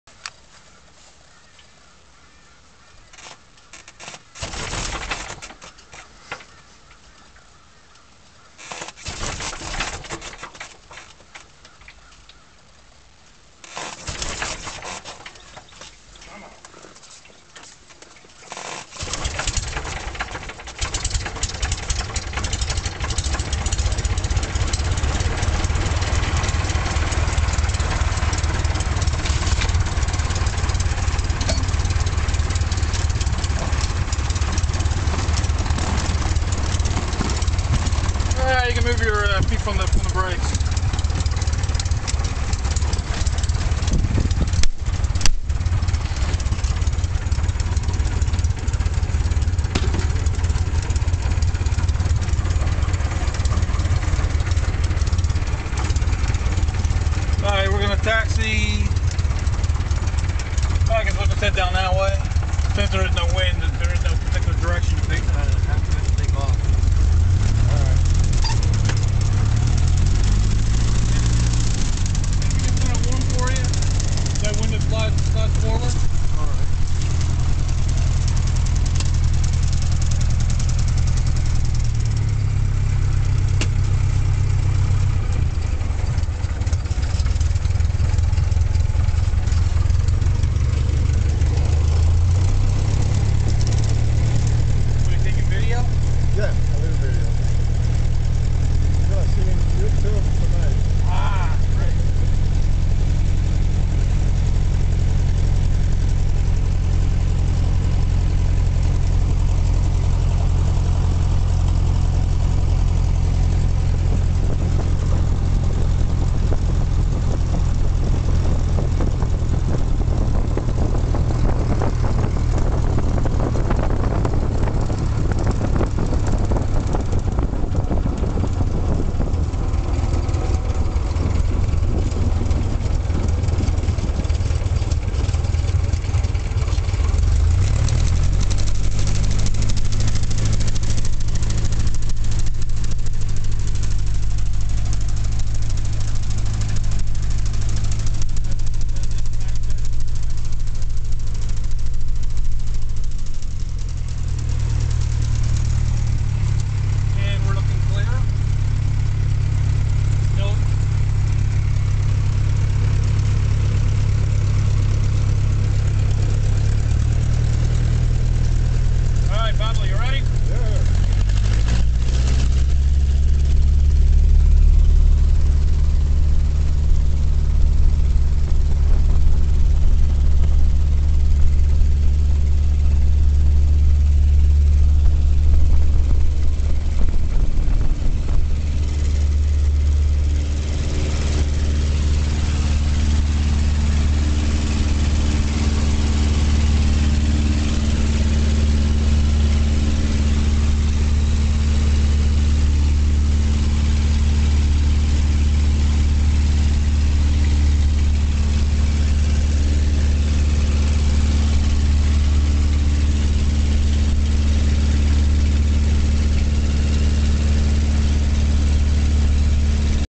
Departure from Quakertown, PA
Location: Quakertown Airport
Hopkins Airlines.
Flying Quakertown-Bally-Quakertown, PA (at lunch-time)